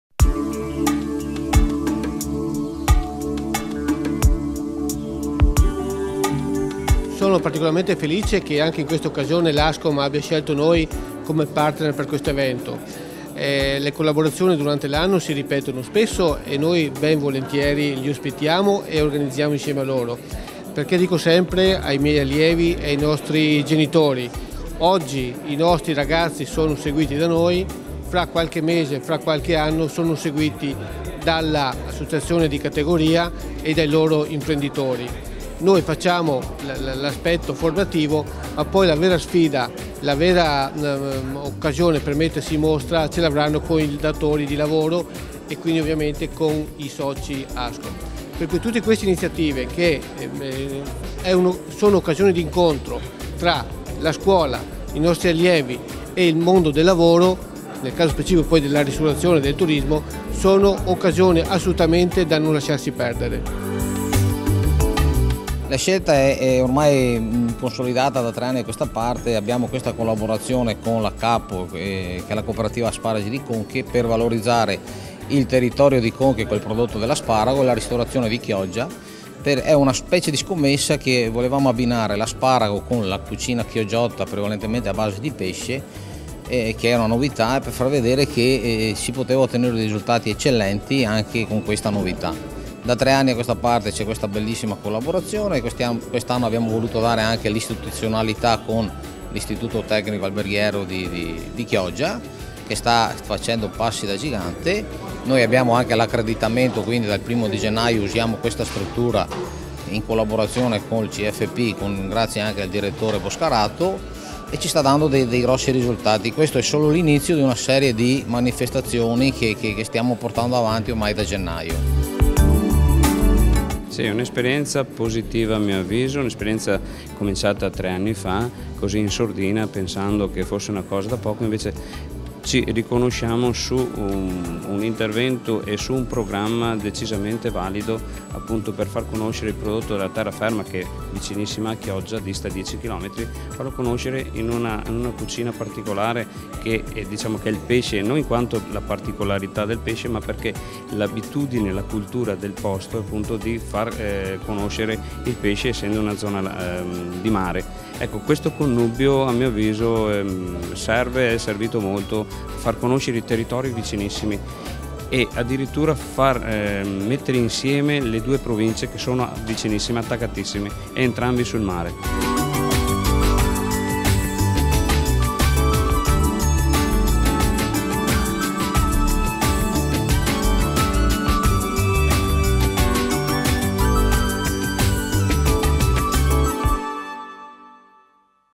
Il Maggio dell'Asparago di Conche a Chioggia
E'iniziata la terza edizione de "Il maggio dell'asparago di Conche a Chioggia". L'iniziativa promossa dall'Ascom e da Gruppo Turismo  è frutto di una collaborazione con la C.A.P.O. di Conche,  produttore dell'asparago di Conche. Per tutto il mese di maggio è previsto l'appuntamento con  i ristorante di Chioggia e Soittomarina che proporanno piatti a base di asparago di Conche ed altri prodotti del territorio.